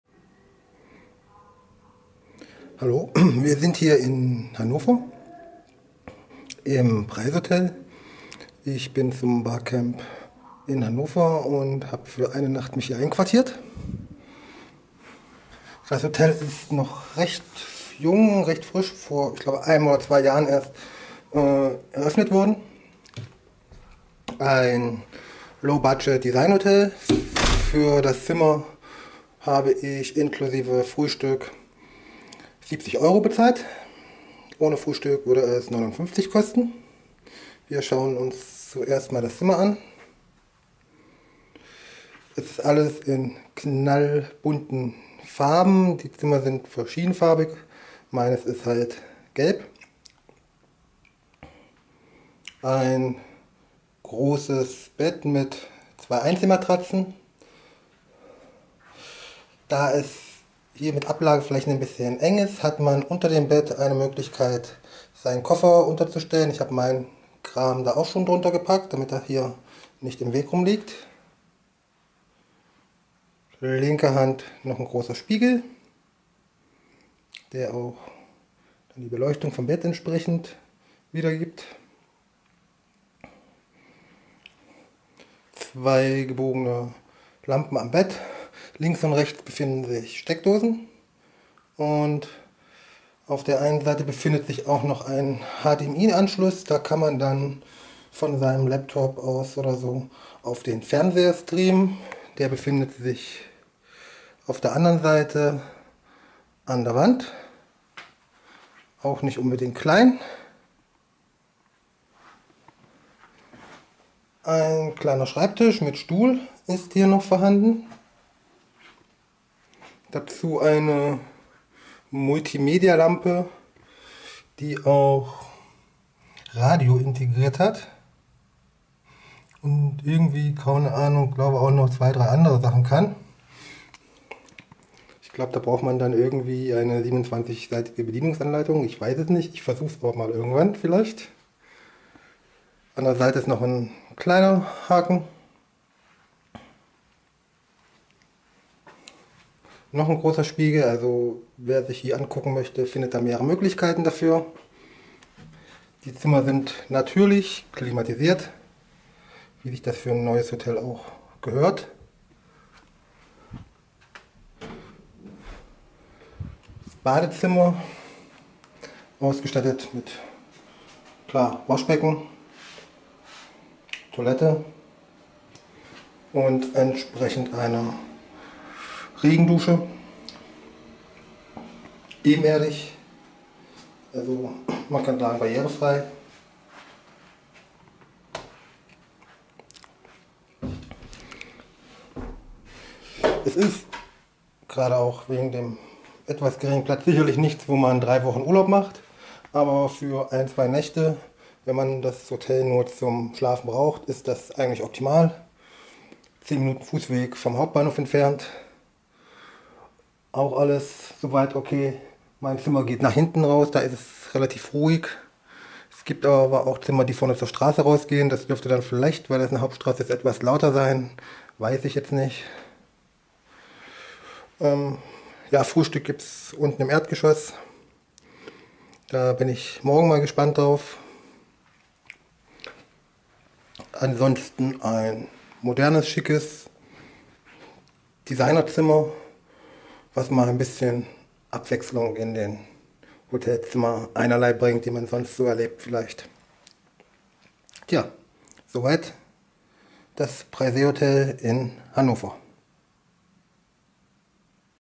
Designhotel Prizeotel Hannover
Einblick in ein Hotelzimmer im Designhotel Prizeotel Hannover.